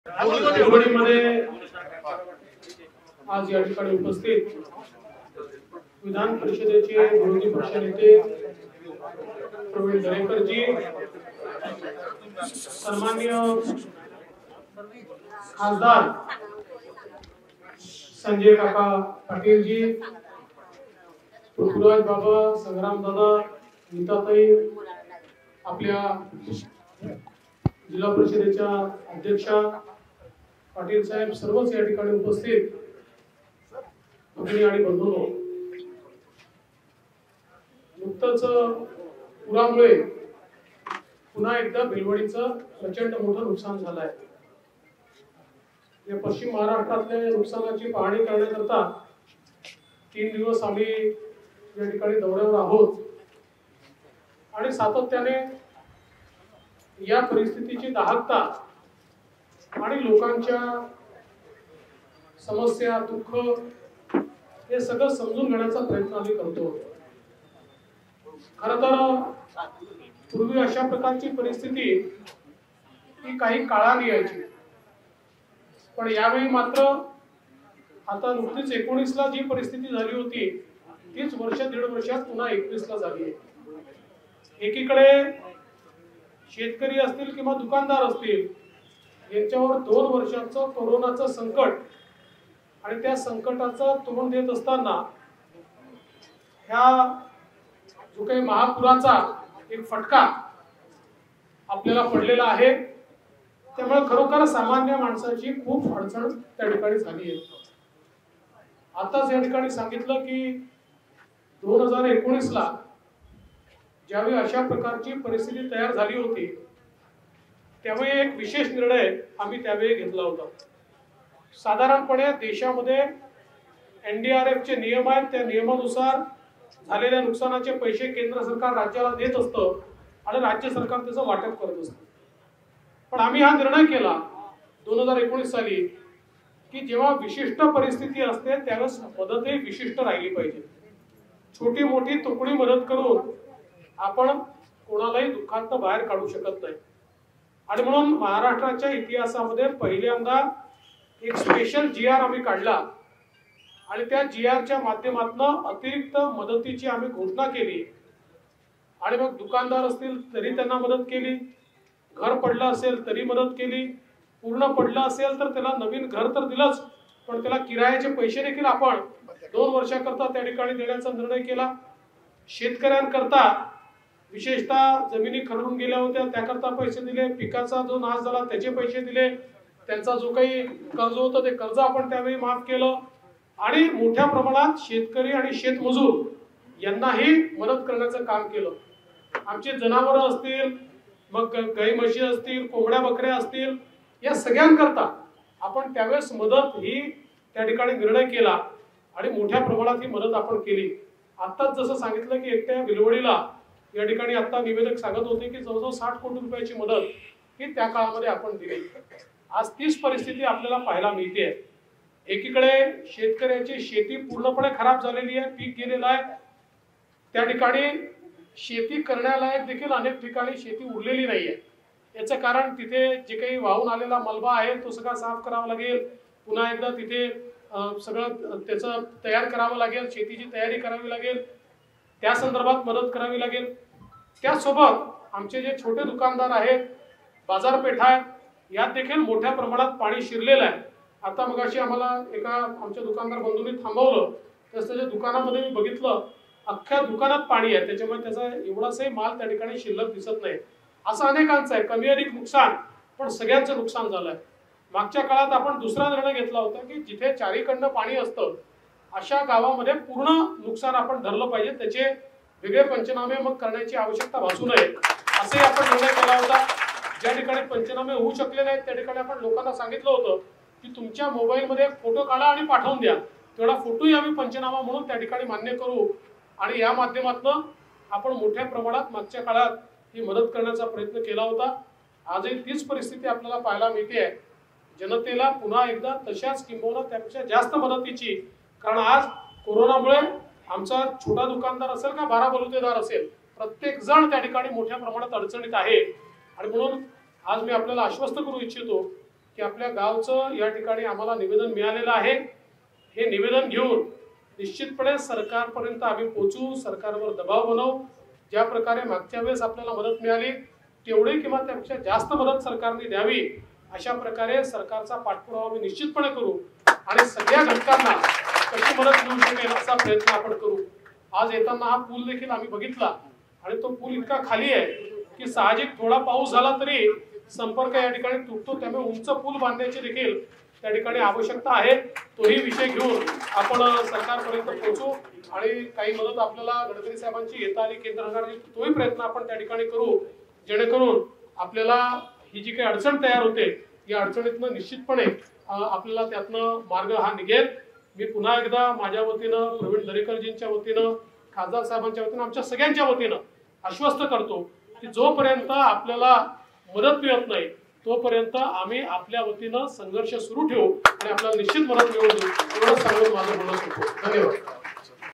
जो पर्यंत भिलवडी पूरग्रस्त भागाला राज्य सरकारकडून मदत मिळत नाही तोपर्यंत संघर्ष सुरु राहणार
जो पर्यंत भिलवडी पूरग्रस्त भागाला राज्य सरकारकडून मदत मिळत नाही तोपर्यंत संघर्ष सुरु राहणार | २९ जुलै, २०२१ । भिलवडी, ता. पलूस, जि.सांगली

0:45 -  भिलवडी, सांगलीमध्ये पुरामुळे मोठे नुकसान
पश्चिम महाराष्ट्रातील पुरामुळे झालेले नुकसान पाहण्याकरता तीन दिवसांचा दौरा. लोकांच्या समस्या, दु:ख समजून घेण्याचा प्रयत्न. शेतकरी आणि दुकानदार दोन वर्षापासून कोरोनाच्या संकटात आणि आता महापुराचा फटका, त्यामुळे सामान्य जनतेची मोठी अडचण.

1:50 - भाजपाच्या काळात भिलवडीला 60 कोटींची मदत मिळाली होती
2019 साली ज्यावेळी पूरस्थिती निर्माण झाली होती तेव्हा भाजपा सरकारने विशेष निर्णय घेतला. केंद्र सरकार एनडीआरएफच्या निकषानुसार राज्याला आप्तकालीन स्थितीत मदत करत असते. पण ज्यावेळी विशिष्ट मदतीची गरज असते त्यावेळी मदतही विशिष्ट जाहीर झाली पाहिजे. महाराष्ट्राच्या इतिहालात पहिल्यांदाच विशेष जीआर काढून भाजपाने शेतकऱ्यांना, दुकानदारांना, शेतमजुरांना मदत केली होती.

3:32 - भिलवडीतील जनतेला राज्य सरकारने जास्तीत जास्त नुकसान भरपाई द्यावी
परिसरात शेतीचे मोठे नुकसान. वाहून आलेला मलबा साफ करावा लागणार. छोट्या बाजारपेठा आणि दुकानांचे मोठया प्रमाणात नुकसान. ज्या ठिकाणी चारही बाजूंनी पाणी होते तो भाग मागील काळात पूर्ण नुकसानाची गृहीत धरला होता, तर जिथे पंचनामे झाले नाहीत, तिथे फोटो काढून पाठवण्यास सांगितले होते. सर्व स्तरातील सर्व घटकांना राज्य सरकारकडून मदत कशी देता येईल यासाठी विरोधी पक्षाचे नेते प्रयत्न करणार. परिसरातील पुलाचा प्रश्नही लवकरात लवकर मार्गी लावणार. जोवर राज्य सरकारकडून मदत मिळणार नाही तोवर भाजपा संघर्ष सुरु ठेवणार.